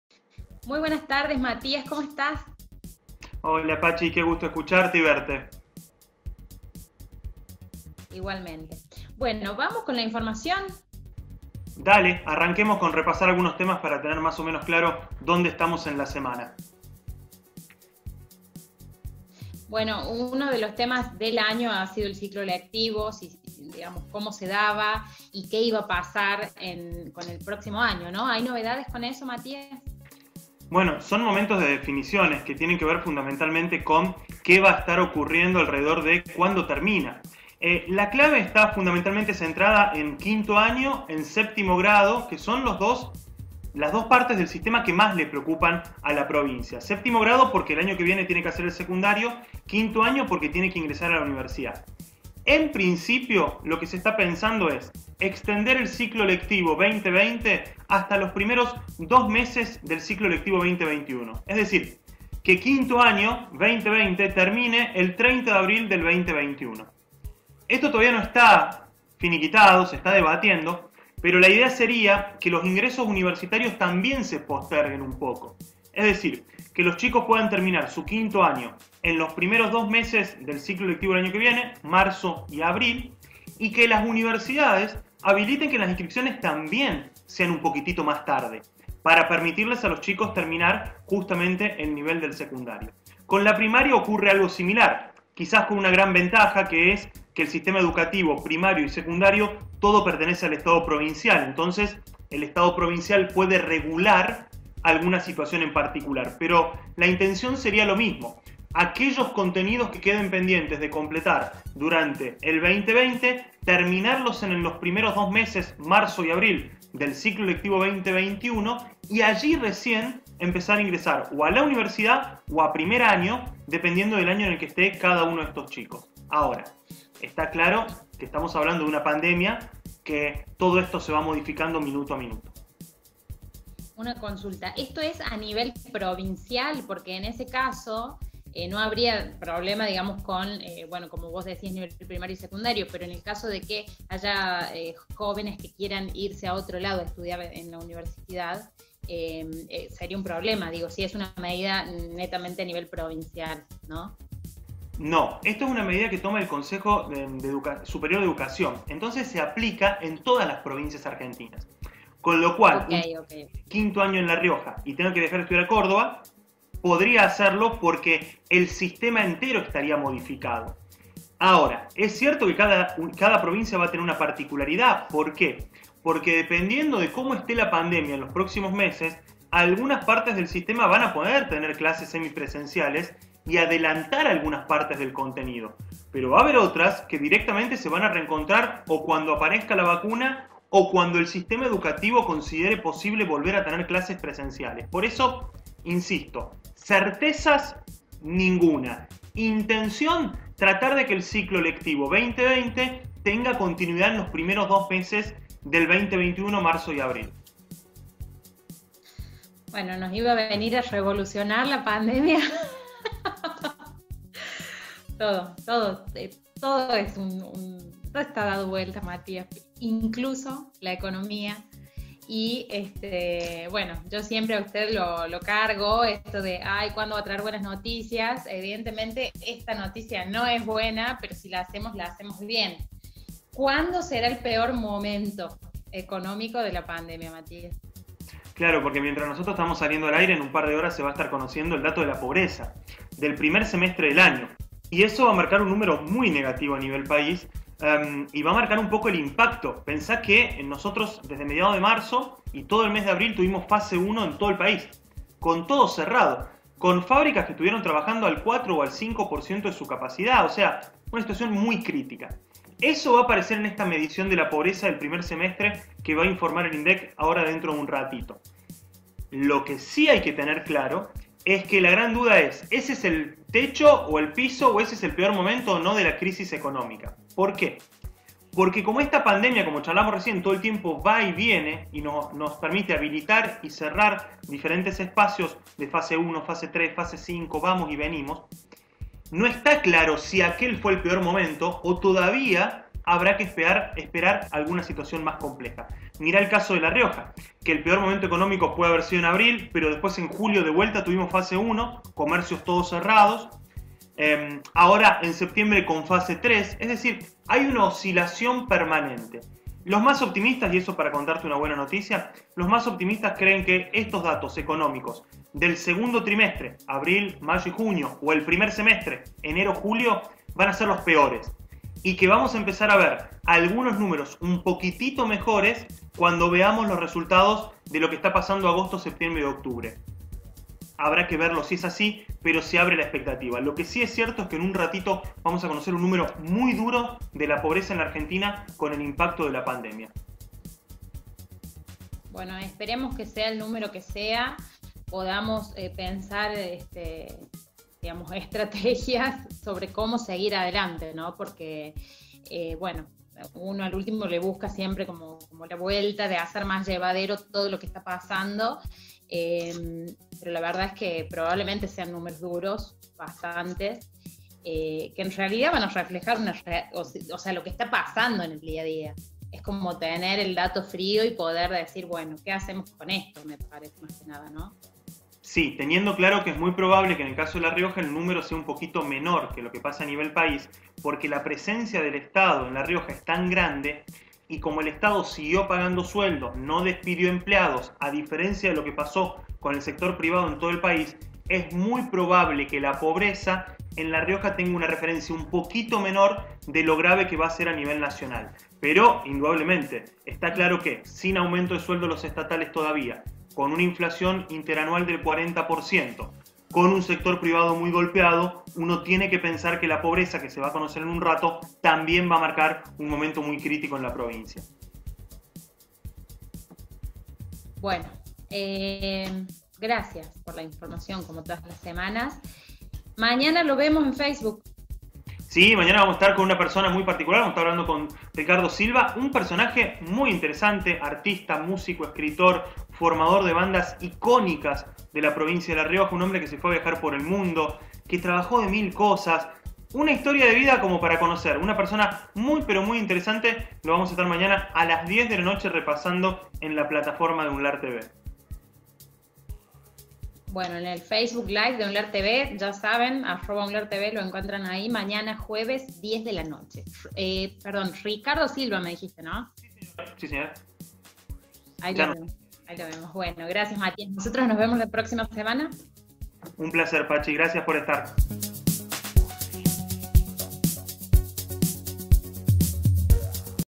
Ciclo lectivo 2021 y los datos del INDEC
Qué pasará con la culminación del ciclo lectivo 2020 y el inicio del 2021.
Cuál es el peor dato económico de la pandemia. Lo analiza Mattías Meragelman .